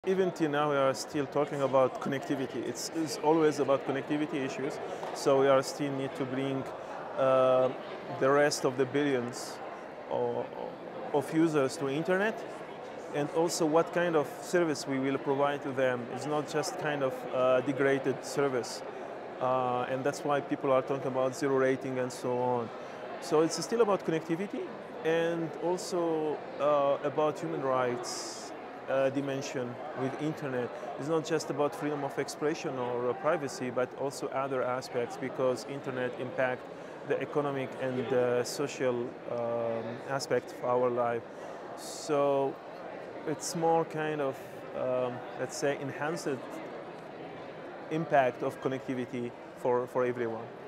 Global IGF 2016: Rafik Dammak on Top Internet Issues
Rafik Dammak, Computer Engineer, NTT Communications, shares his answer to the question, “What are the most difficult Internet challenges, fears or issues that must be addressed to assure the best future for all?” Recorded at the Global Internet Governance Forum 2016 in Guadalajara, Mexico, by the Imagining the Internet Center, an initiative of Elon University, North Carolina, USA.